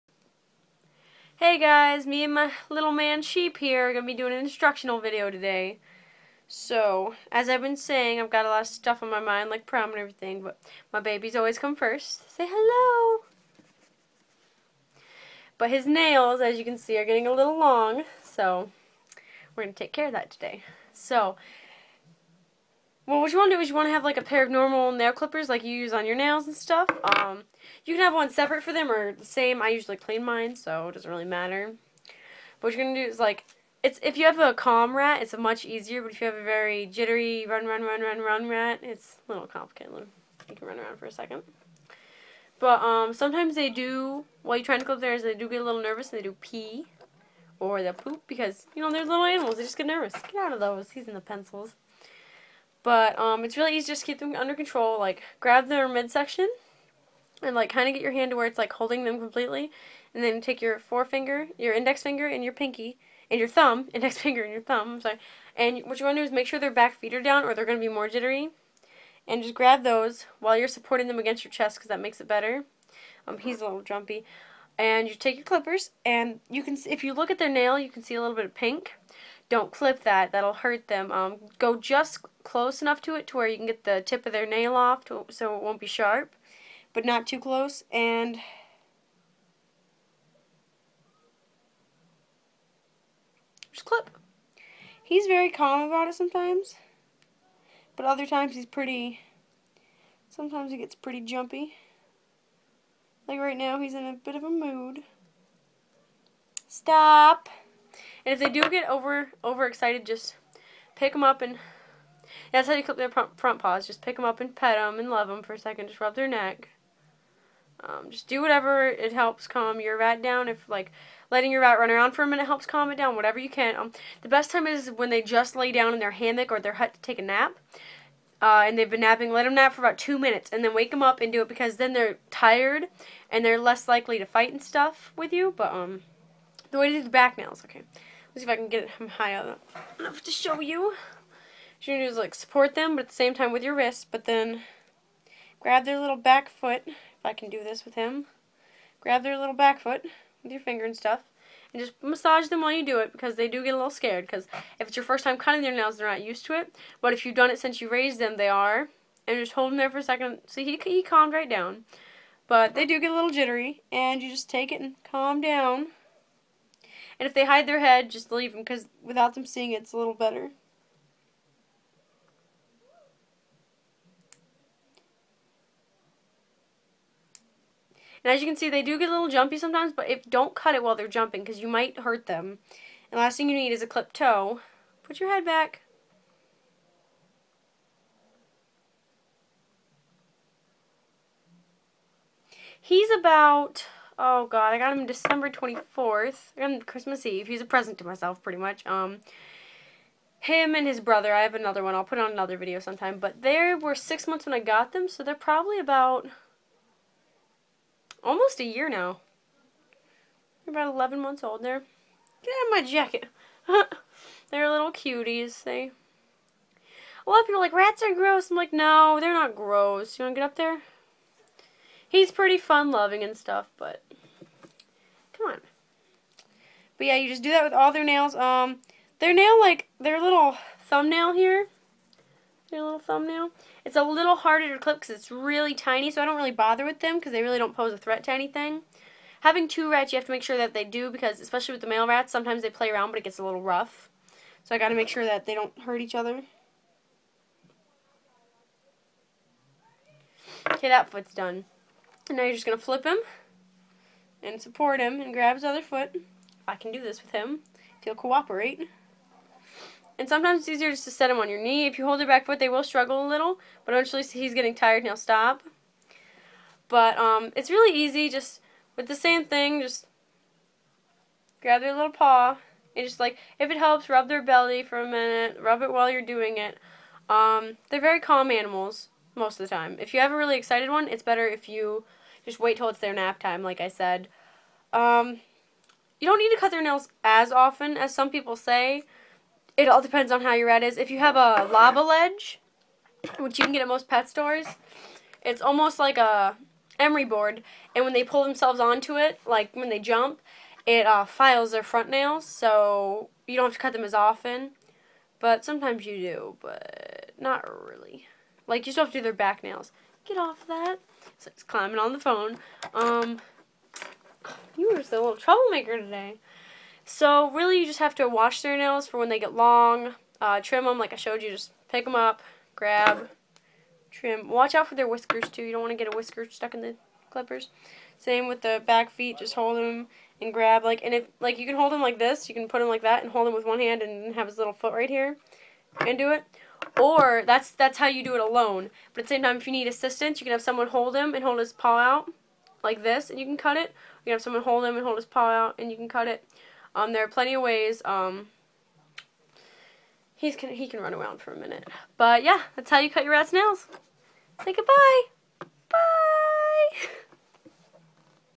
How to Clip a Rat's Nails
Just me and my baby, Sheep. His little nails were getting long so it was time to clip them. Any questions just ask in the comments. He's an 11 month old male rat. Using normal nail clippers that you can find almost anywhere work, be safe and careful.